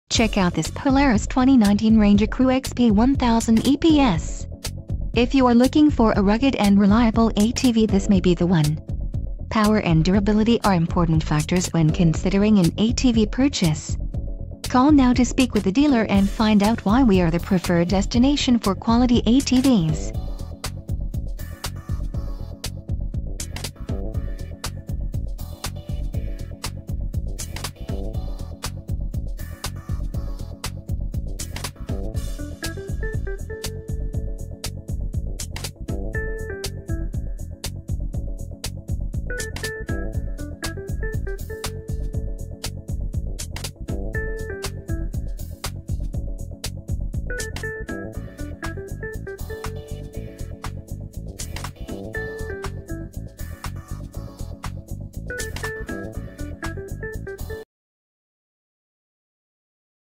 2019 Polaris® Ranger Crew® XP 1000 EPS
2019 Polaris and #174; Ranger Crew and #174; XP 1000 EPSTHE NEW STANDARD FOR 6-PASSENGER UTILITY
  and nbsp;

  MORE RUGGED. MORE REFINED. MORE RANGER. and #8482;

 The all-new RANGER CREW and #174; XP 1000 brings added passenger and payload capacity to the world and #8217;s Hardest Working, Smoothest Riding and #174; utility side-by-side. It and #8217;s more rugged, more refined, and fueled by over one-hundred owner-inspired improvements.
Features may include:
  BEST-IN-CLASS BY ANY MEASURE

Get more done with the industry and #8217;s most capable six-person utility side-by-side.